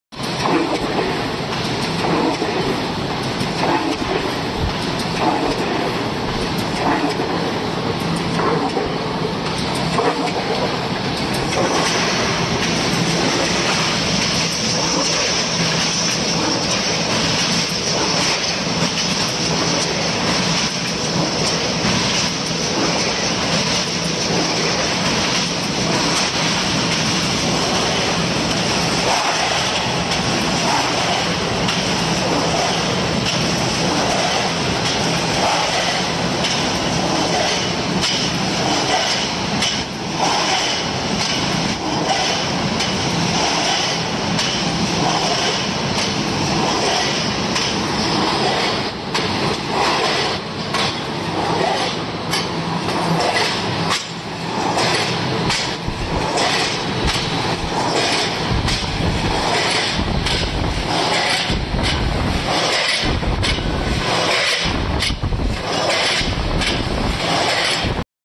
Automatic single slitter 2
Automatic Single Slitter

Including sheet feeder, slitter, sheet collector and feeding erection platform
With 6 pairs of carbide cutters
Min. sheet width: 40mm
Max. sheet width: 1300mm
Max. sheet thickness: 0.5mm
Power: about 3Kw
Total weight: about 2600Kg

Castle Chen
Delicacy Packaging Machinery Co., Ltd.